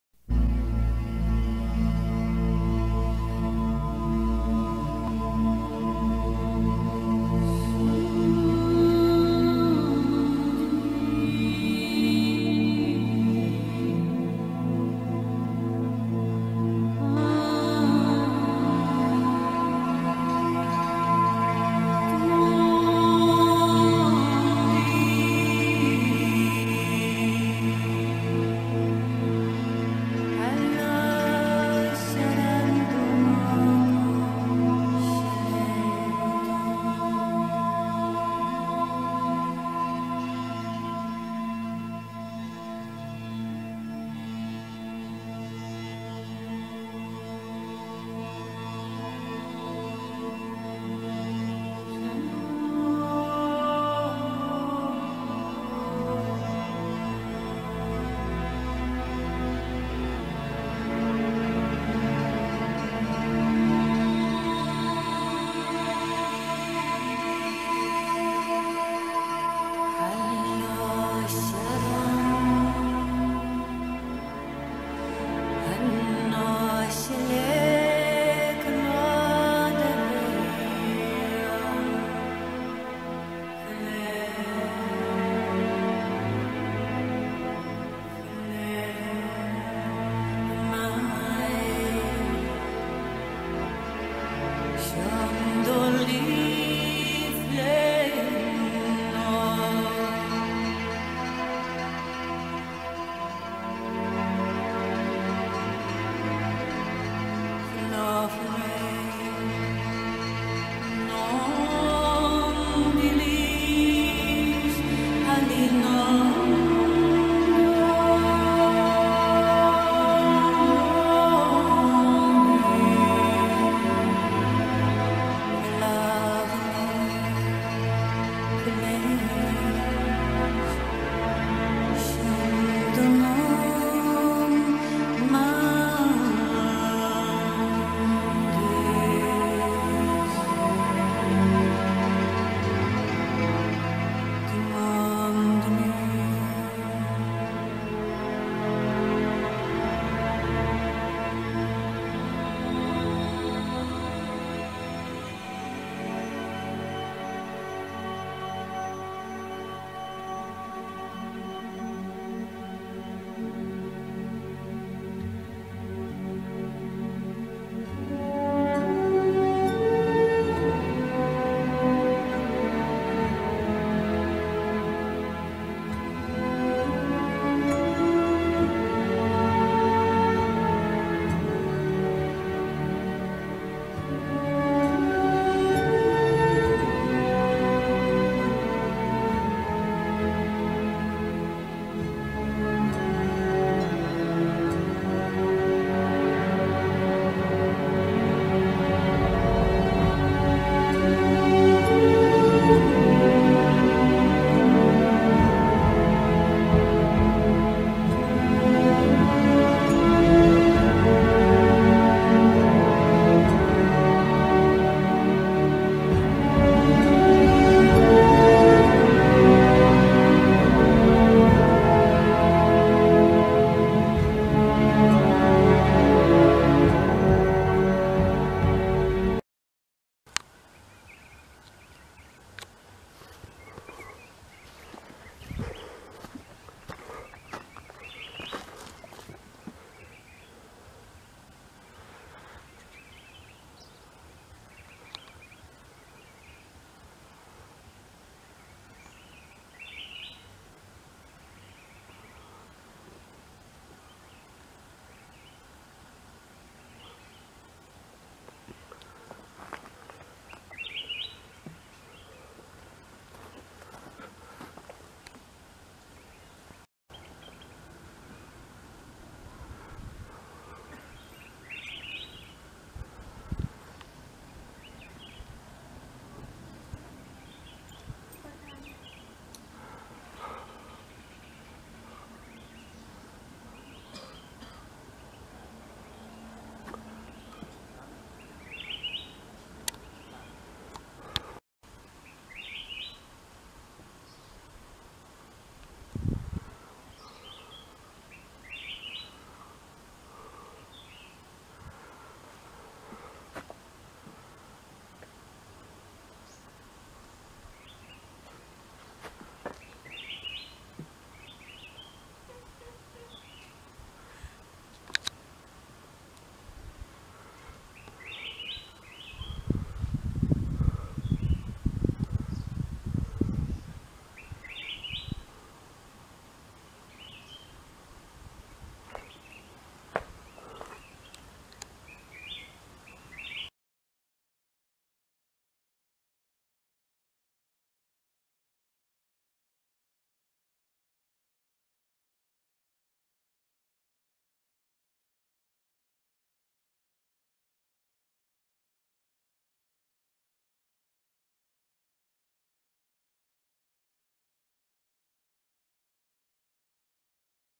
Cambodia: Phnom Bok - Photograph Slideshow
Photos and videos taken in February 2022. Greener photos at the end taken in September 2014.